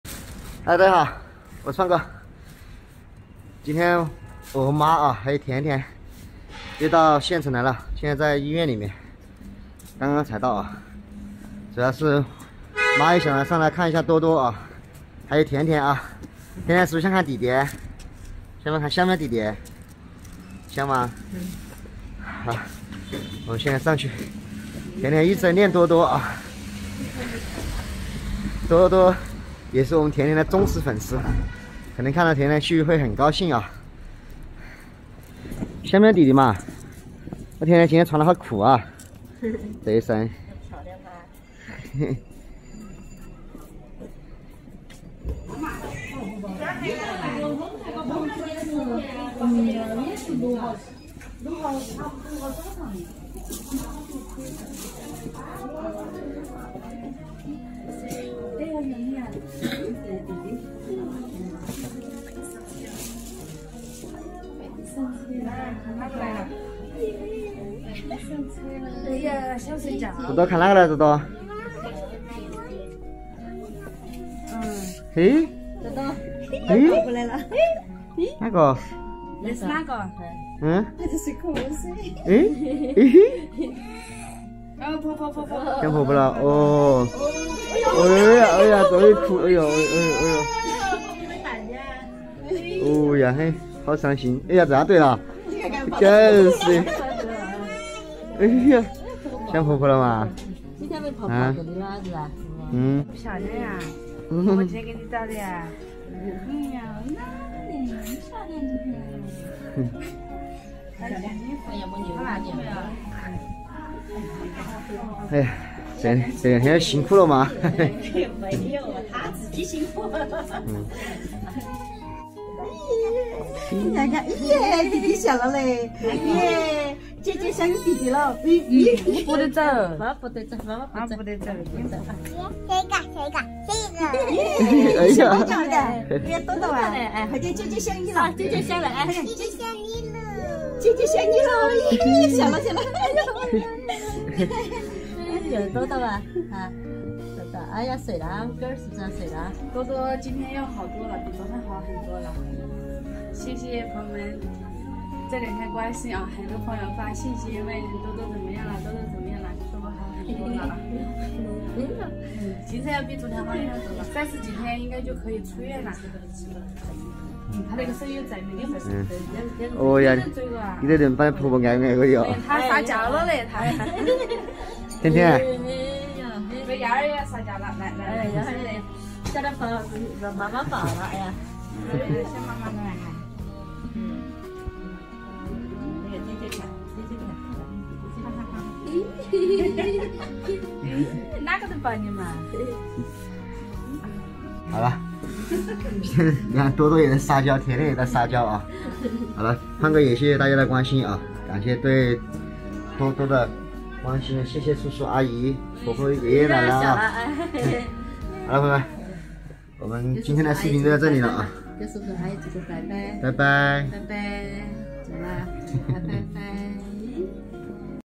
多多住院媽和甜甜去縣城看望，生病的多多一見到婆婆就開始撒嬌，好心疼【菊子的鄉味】
多多本來好好的，結果一看到婆婆就哭了，這麼小就懂得撒嬌~